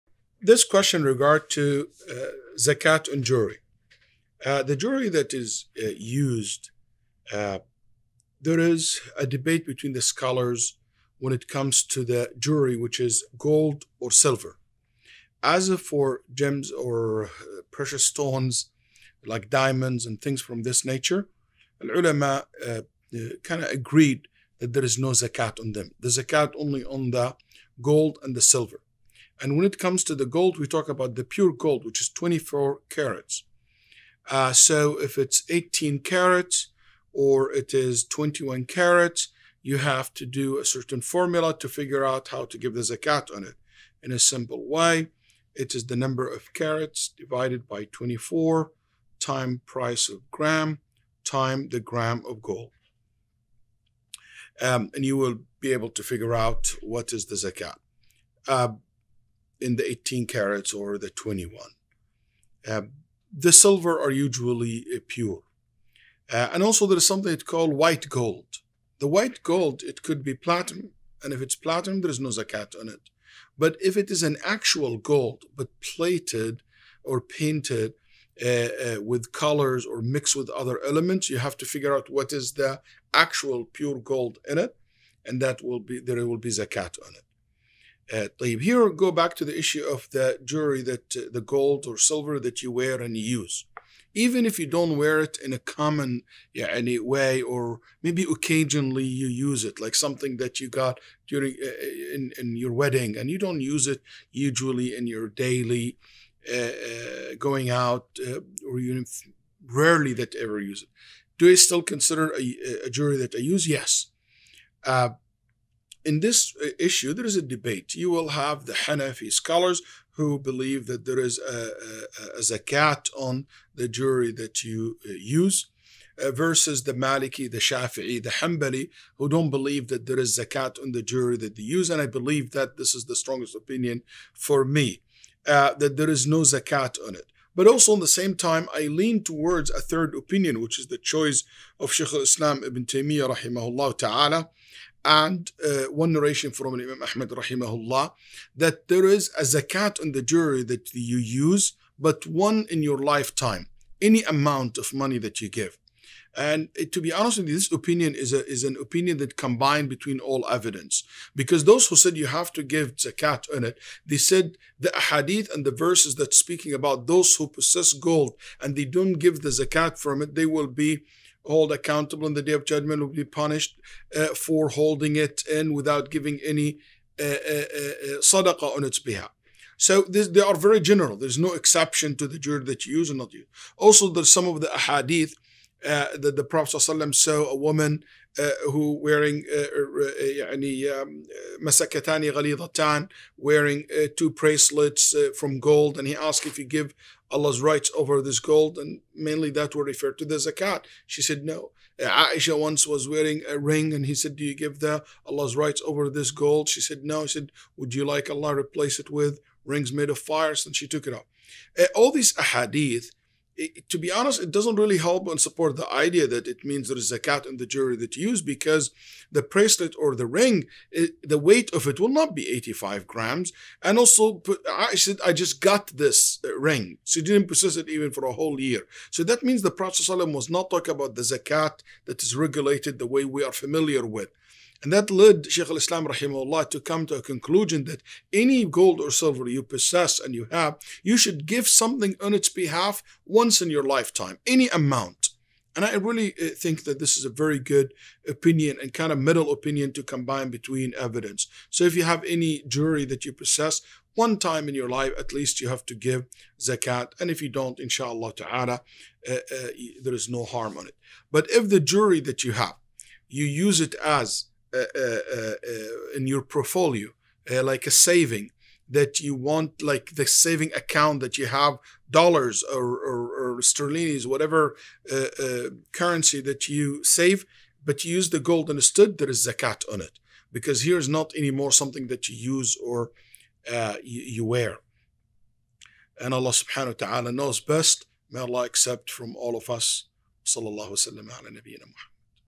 Zakat on Jewelry | Waleed Basyouni | Detox Ramadan Fiqh
How much zakat do I give on my jewelry?